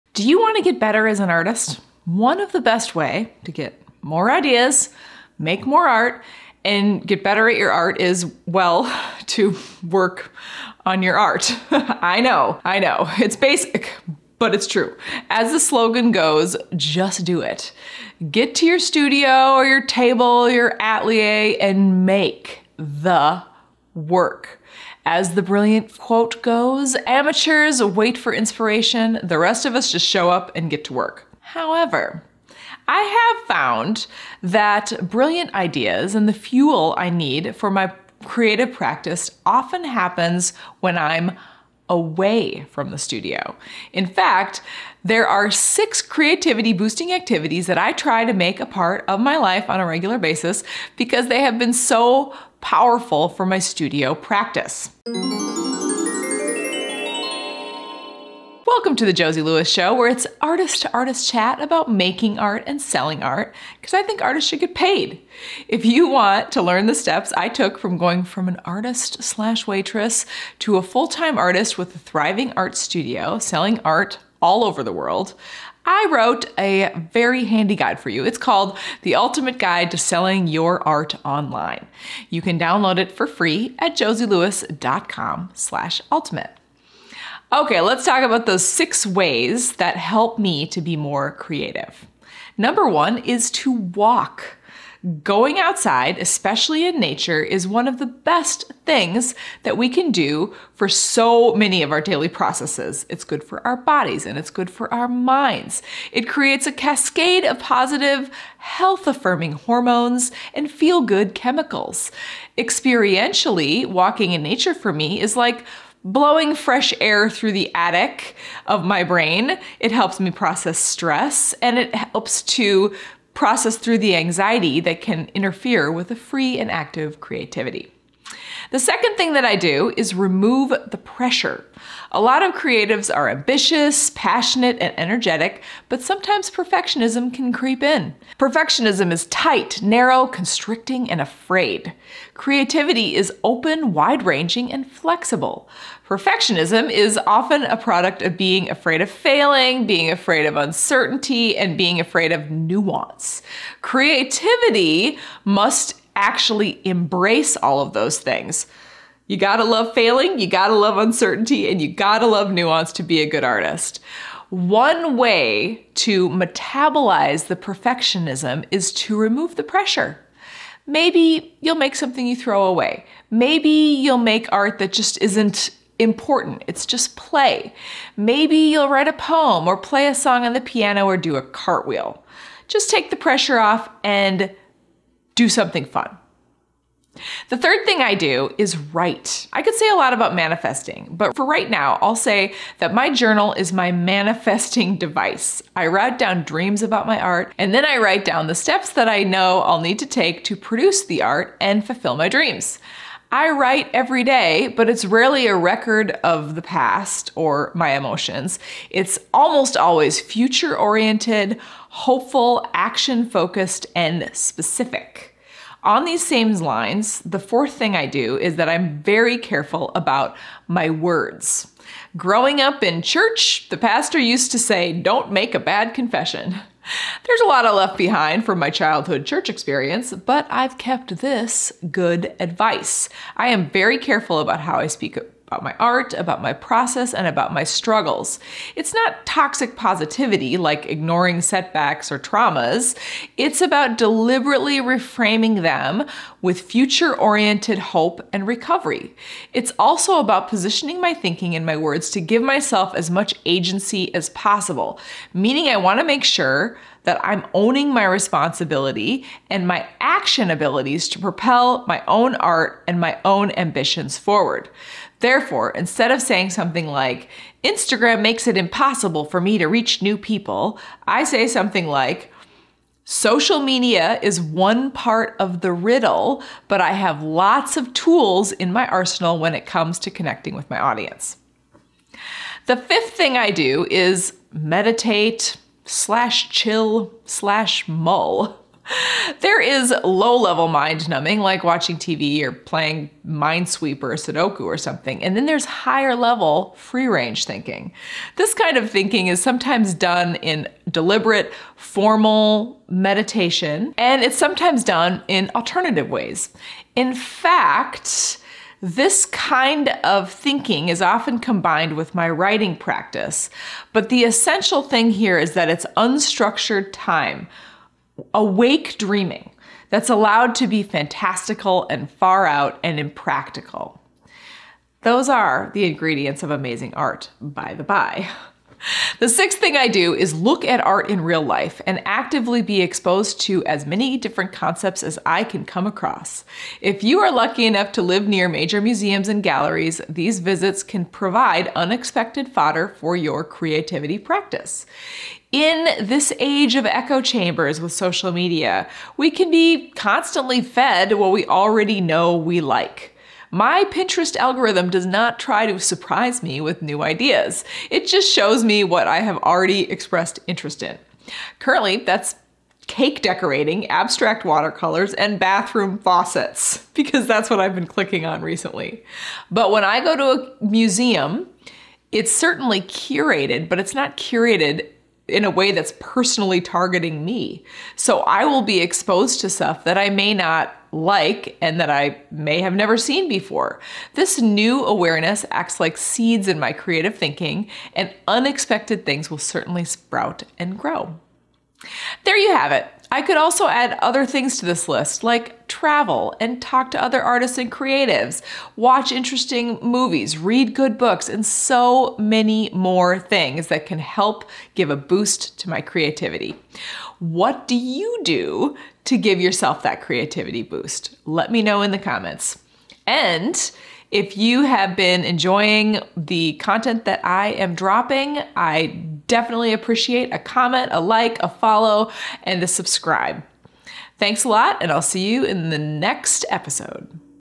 What You NEED To Boost Your Creativity
One of the best ways to get more ideas, make more art, and get better at your art is, well, to work on your art!  As the slogan goes, “just do it”.  Get to your studio or your art table or your atelier and make.  The. Work.  As the brilliant quote goes “amateurs wait for inspiration. The rest of us show up and get to work." However, I have found that brilliant ideas and the fuel I need for my creative practice often happens AWAY from my studio.  In fact, there are 7 creativity boosting activities that I try to make a part of my life on a regular basis because they have been so powerful for my studio practice.